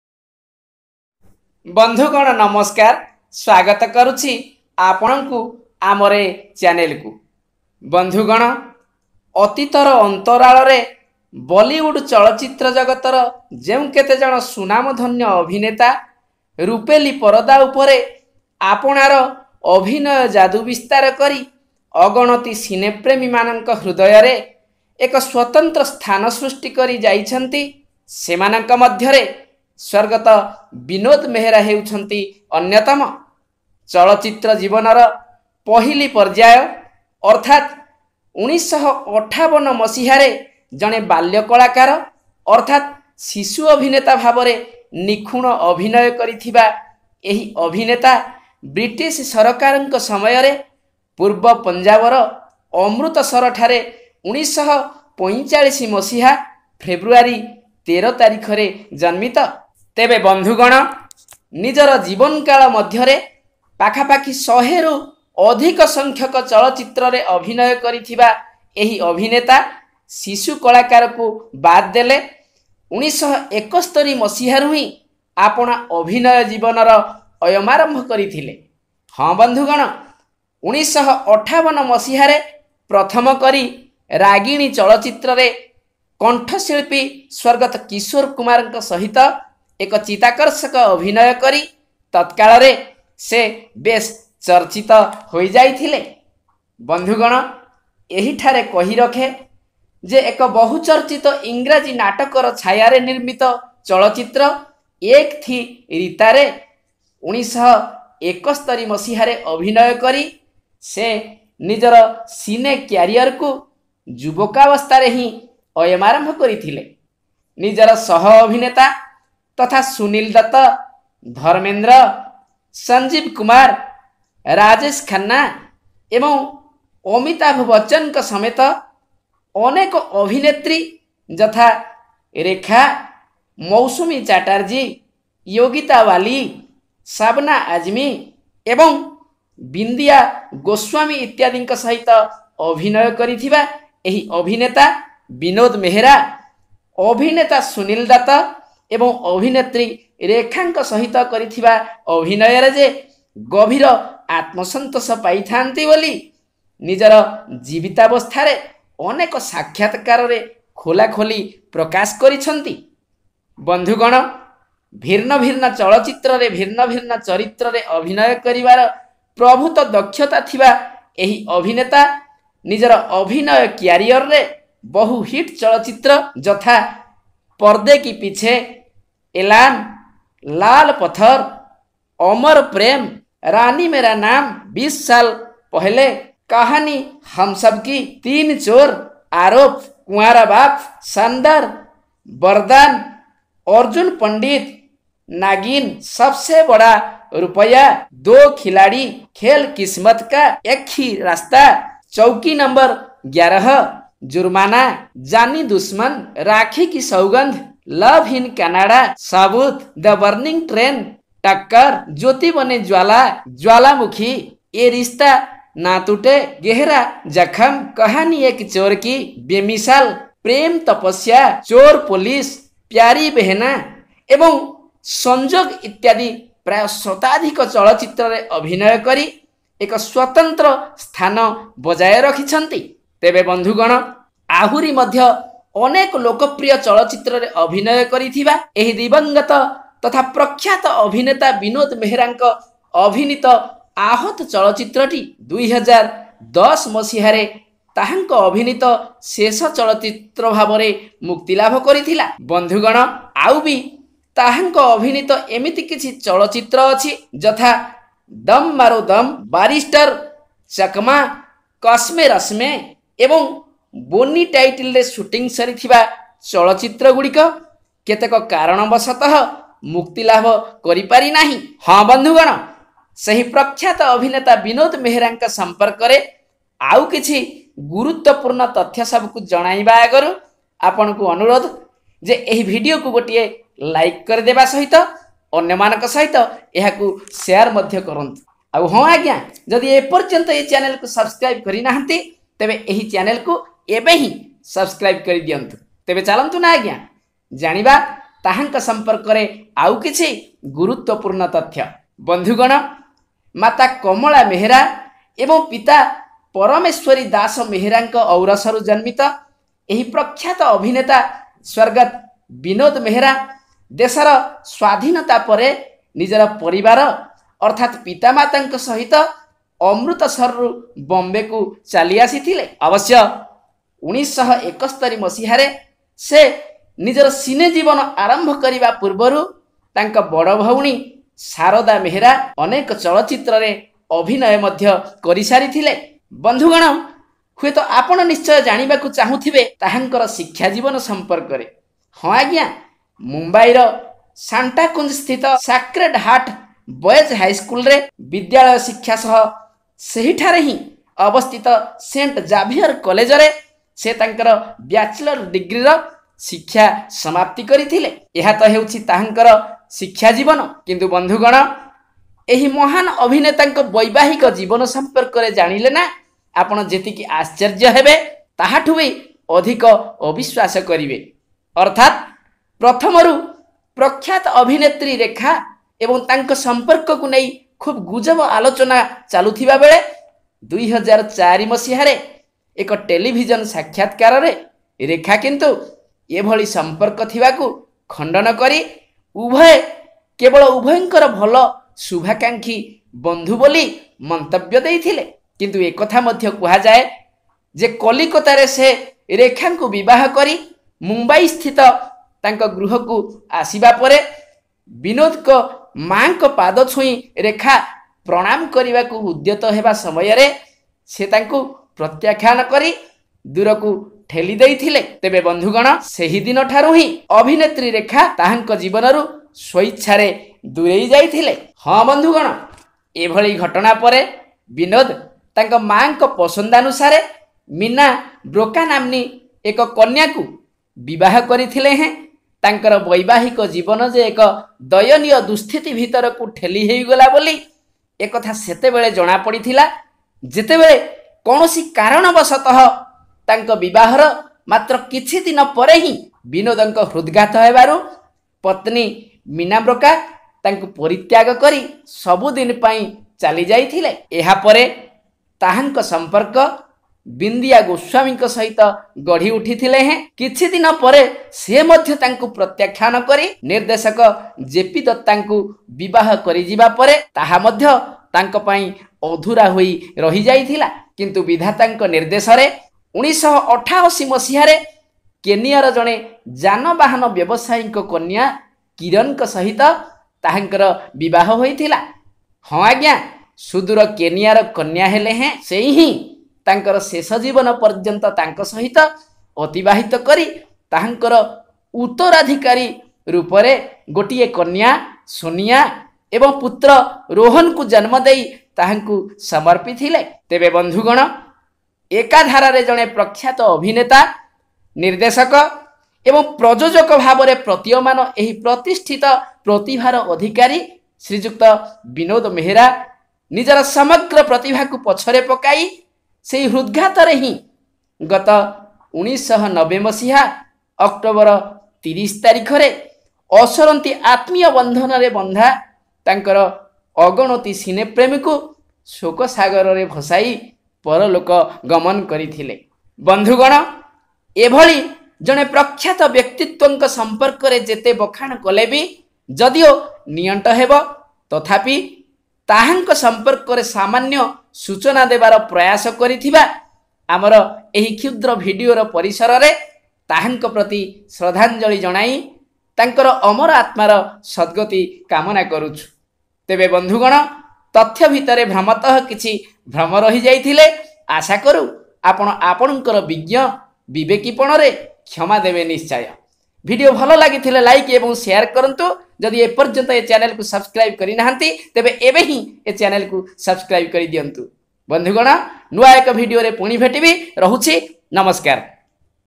Biography of Bollywood Actor Director Producer Vinod Mehra ଅଭିନେତା ବିନୋଦ ମେହେରାଙ୍କ ଜୀବନ କଥା
ପ୍ରଖ୍ୟାତ ବଲିଉଡ୍ ଚଳଚ୍ଚିତ୍ର ଅଭିନେତା ନିର୍ଦ୍ଦେଶକ ଏବଂ ପ୍ରଯୋଜକ ସ୍ବର୍ଗତ ବିନୋଦ ମେହେରାଙ୍କ ଜୀବନ କଥାକୁ ନେଇ ପ୍ରସ୍ତୁତ ଏହି ଭିଡିଓକୁ ଦେଖି ଏହାକୁ ଲାଇକ ଏବଂ ସେୟାର କରିବା ସହିତ ଏପର୍ଯ୍ୟନ୍ତ ଏ ଚ୍ୟାନେଲକୁ ସବ୍ସକ୍ରାଇବ ନକରିଥିଲେ ଏହି ଚ୍ୟାନେଲକୁ ସବ୍ସକ୍ରାଇବ କରିଦେବାକୁ ଅନୁରୋଧ l ଧନ୍ୟବାଦ